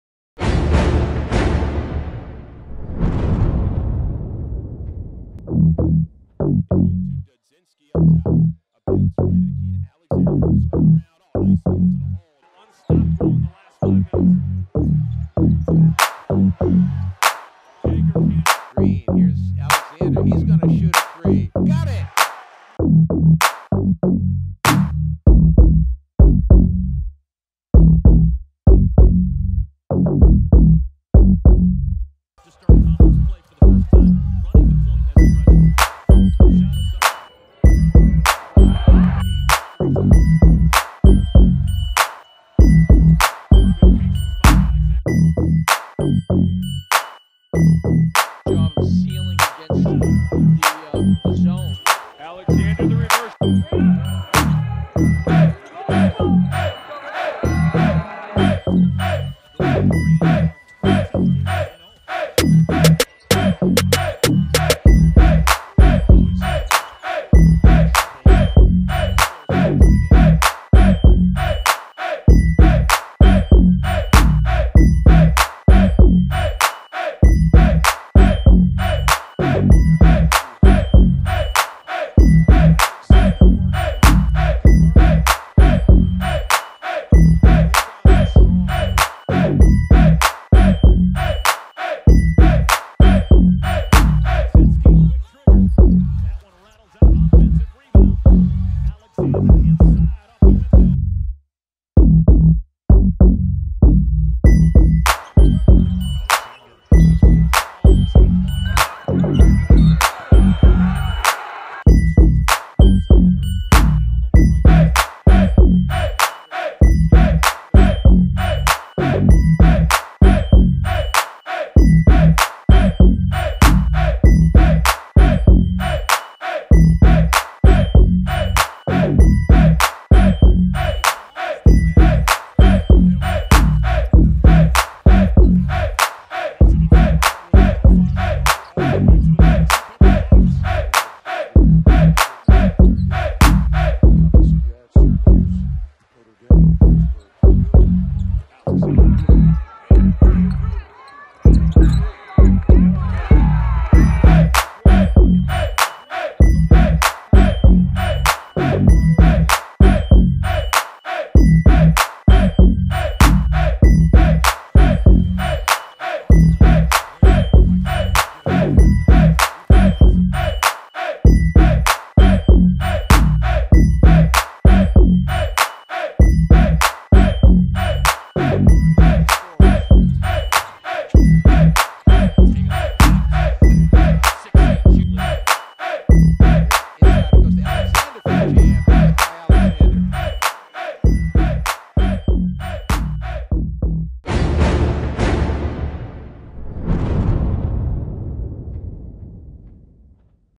Men's Basketball Summer Segment: Malachi Alexander (Holy Cross Athletics)
As we look ahead during these summer months to the start of the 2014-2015 basketball season, check out Malachi Alexander's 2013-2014 highlights!